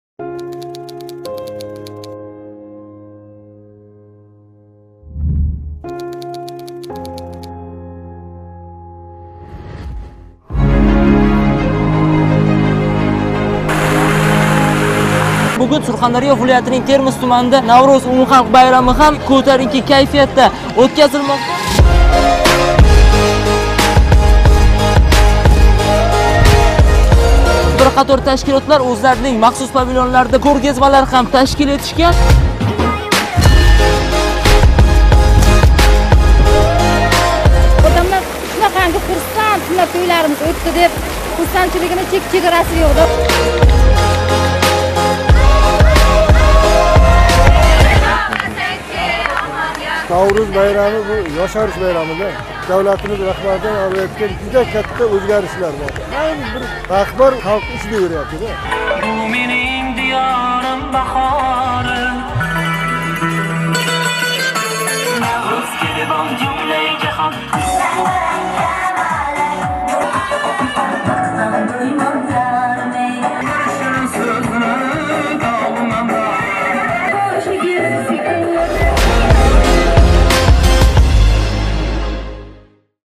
Термиз туманида "Наврўз" умумхалқ байрами сайллари бўлиб ўтди.
Бугун Термиз туманидаги "Марварид" дам олиш масканида янгиланиш ва яшариш байрами, шарқ мамлакатларининг янги йили бўлмиш "Наврўзи олам" байрами катта шодиёналар билан нишонланди. Миллийлигимиз акс этган кўргазмалар, санъаткорлар ижросидаги куй-қўшиқлар тадбир иштирокчиларига байрамона кайфият улашди.

Шунингдек, 48 нафар кам таъминланган, ижтимоий кўмакка мухтож ва боқувчисини йўқотган оила фарзандларининг суннат тўйлари ўтказилиб, уларга совғалар топширилди.

 Сурхондарё вилояти ҳокимлиги Матбуот хизмати